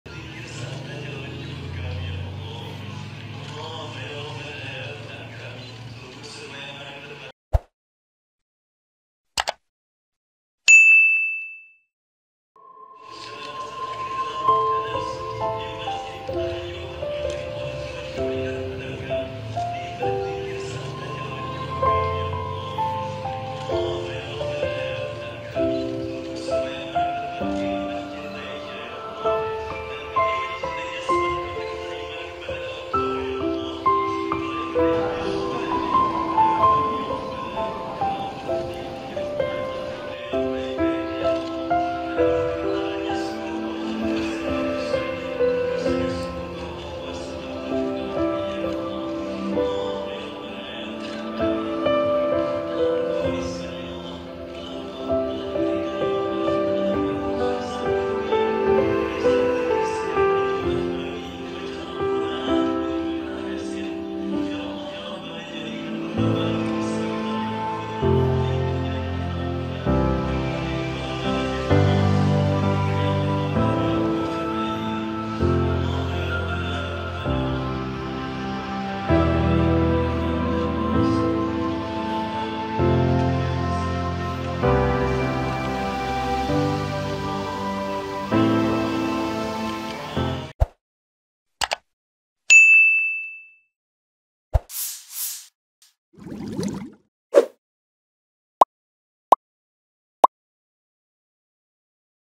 Ridwan Kamil Menangis Saat Salatkan Jenazah Walikota Bandung di Pendopo Bandung
Gubernur Jawa Barat Ridwan Kamil menangis saat berdoa setelah menyalatkan Jenazah Oded di Pendopo Bandung.

Walikota Bandung, Oded M Daniel  meninggal dunia saat melakukan salat sunah di Masjid Muhammadiyah.

Terlihat juga Mantan Wakil Presiden Jusuf Kalla di barisan pertama makmum.

Dalam video terlihat Ridwan Kamil, mengenakan batik biru dan peci hitam.

Video terkini dan terbaru..